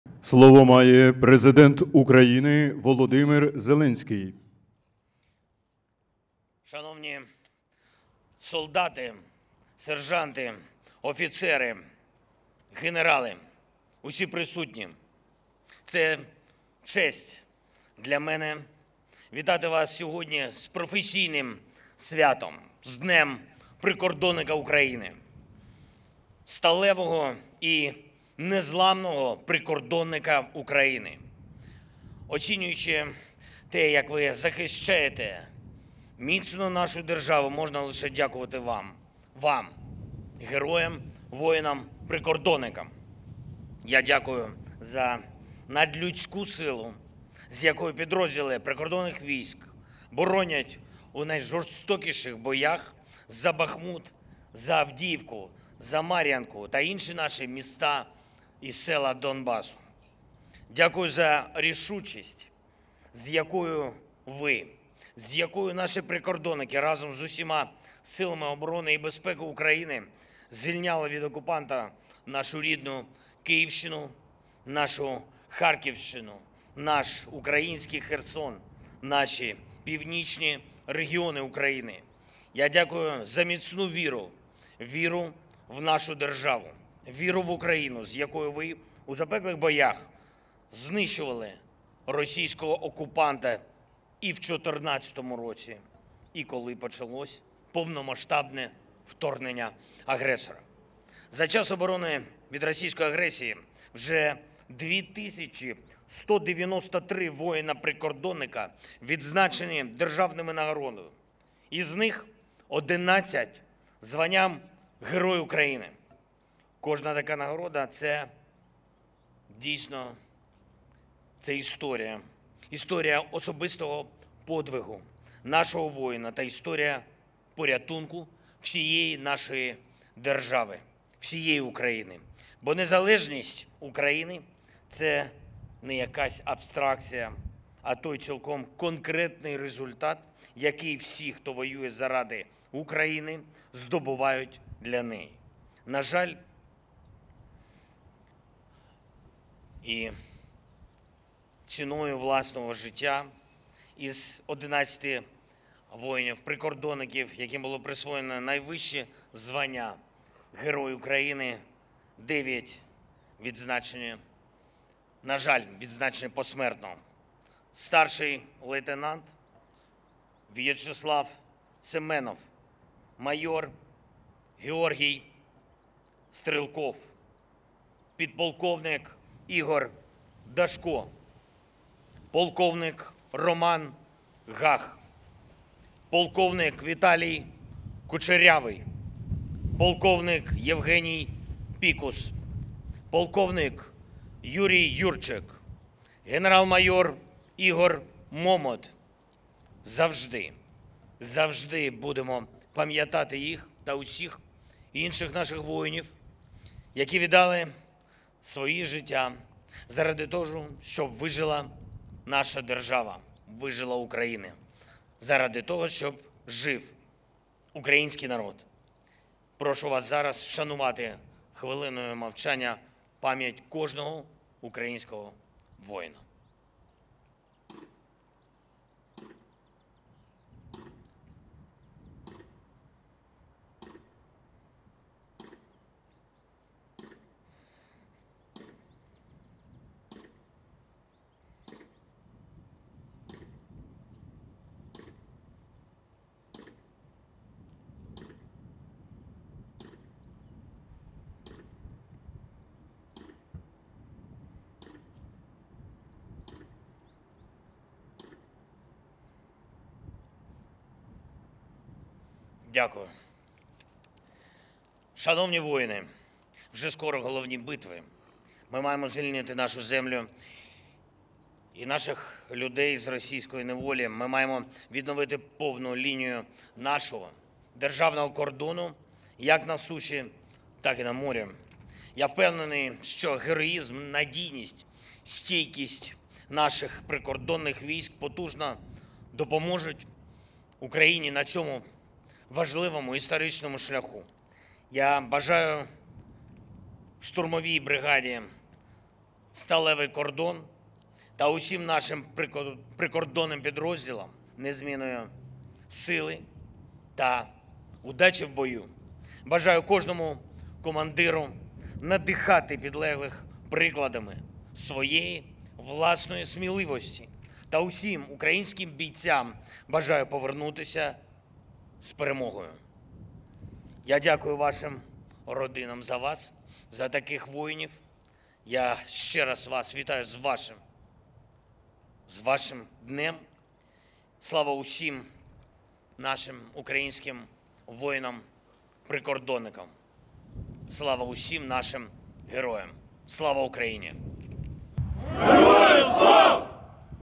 "Я дякую за надлюдську силу" - Зеленський привітав українських прикордонників
«Я дякую за надлюдську силу, з якою підрозділи прикордонних військ боронять у найжорстокіших боях Бахмут, Авдіївку, Марʼїнку та інші наші міста і села Донбасу. Дякую за рішучість, з якою ви, наші прикордонники, разом із усіма силами оборони і безпеки України звільняли від окупанта нашу Київщину і Харківщину, наш український Херсон, наші північні регіони України. Дякую за міцну віру – в нашу державу, в Україну, – з якою ви у запеклих боях знищували російського окупанта і в 14-му році, і коли почалось повномасштабне вторгнення агресора. Уже скоро – головні битви. Ми маємо звільнити нашу землю і наших людей з російської неволі. Ми маємо відновити повну лінію нашого державного кордону – як на суші, так і на морі. Я впевнений, що героїзм, надійність та стійкість наших прикордонних військ потужно допоможуть Україні на цьому важливому історичному шляху. Я бажаю штурмовій бригаді «Сталевий кордон» та всім нашим прикордонним підрозділам незмінної сили та удачі в бою!»

Володимир Зеленський привітав українських прикордонників із професійним святом.